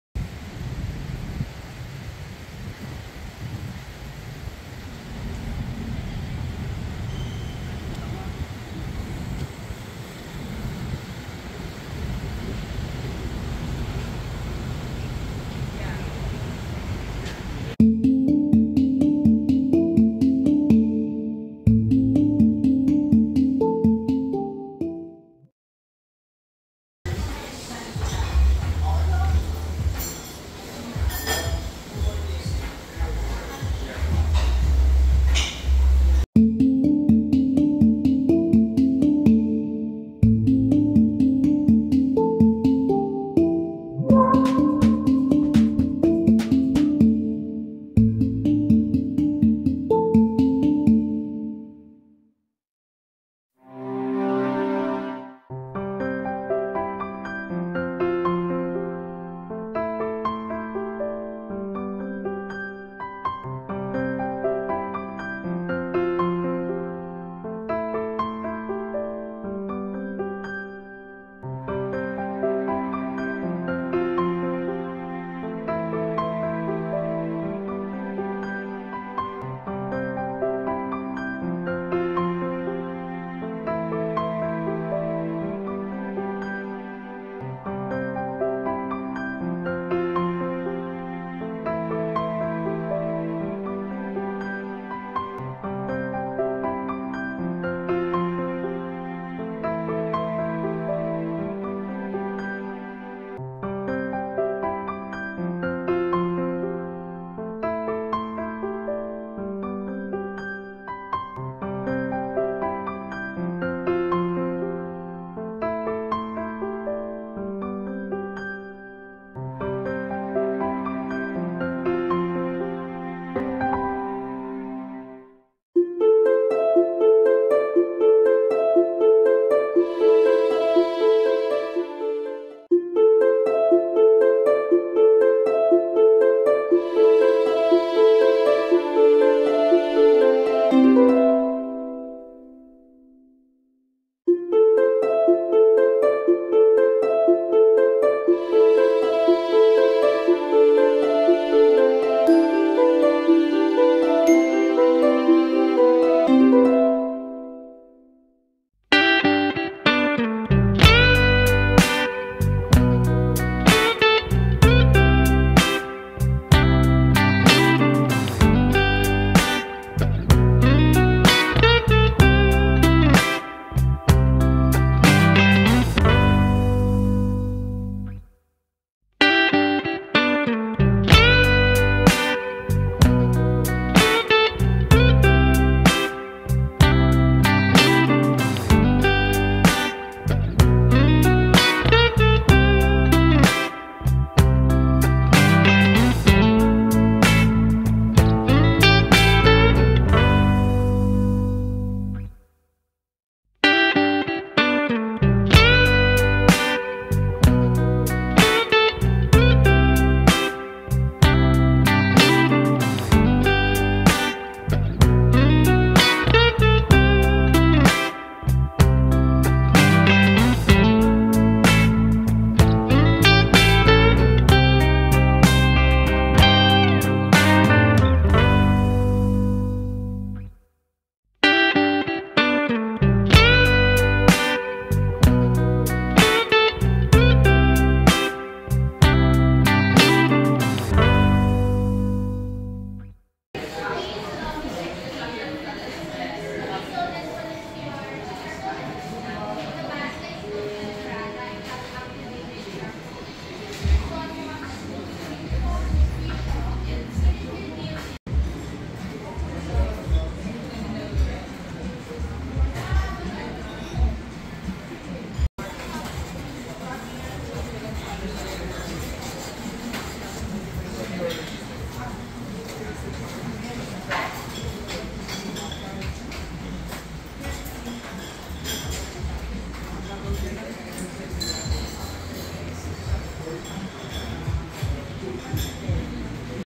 Test Kitchen, Makati by Chef Josh Boutwood ( Philippines 🇵🇭). Trying their 'Tasting Menu'
Makati is one of the main business and lifestyle hubs of Metro Manila, Philippines. In this video we are trying the ' Tasting Menu' of The Test Kitchen by Chef Josh Boutwood in Makati City.

The Test Kitchen is a part of the Bistro Group which has many culinary formats. Chef Josh Boutwood is the Head Chef of the group and oversee restaurants like Test Kitchen, Ember and Savage directly.  But Bistro Group has many more brands under their wings.

We show you the seven course meal of The Test Kitchen, Makati